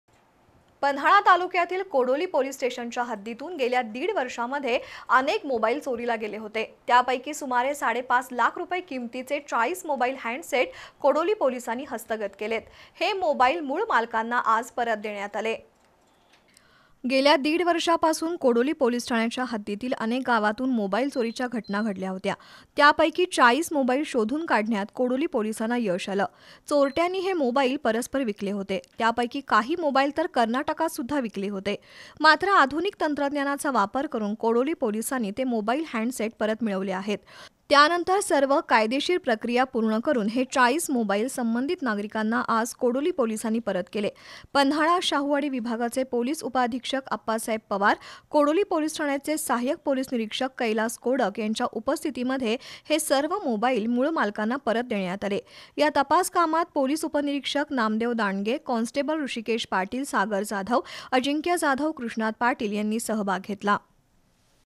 चोरीला गेलेले सुमारे साडेपाच लाख रुपये किमतीचे ४० मोबाईल हॅण्डसेट मुळ मालकांना दिले परत
कोडोली पोलिस स्टेशनच्या हद्दीतून गेल्या दीड वर्षांमध्ये चोरीला गेलेले सुमारे साडेपाच लाख रुपये किमतीचे ४० मोबाईल हॅण्डसेट मुळ मालकांना दिले परत